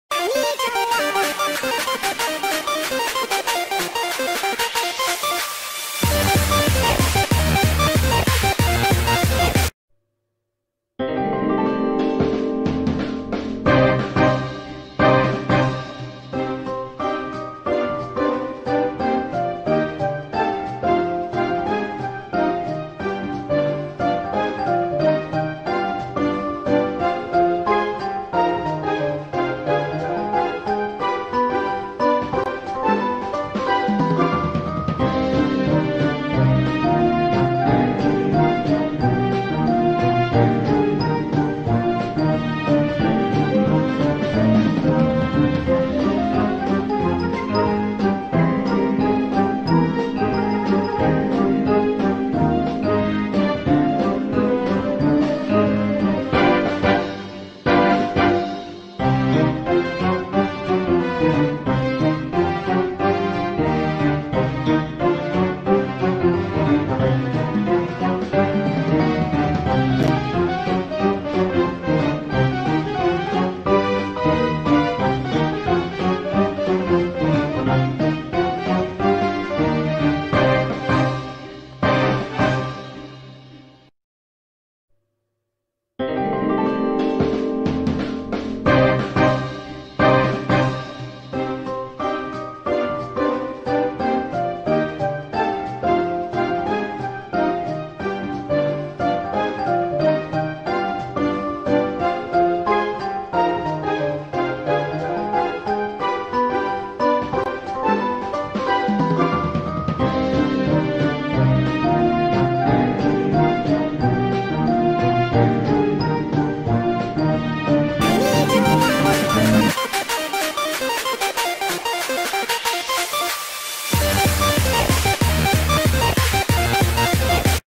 ORANGE Exotic Pet DNA Vial | Pet Scorpions | October Update Prediction/Edit
It's been a while since the last Exotic Pet DNA Vial was released, perhaps it's time that another gets released. I make an edit/prediction for an Orange DNA hatching Pet Scorpions.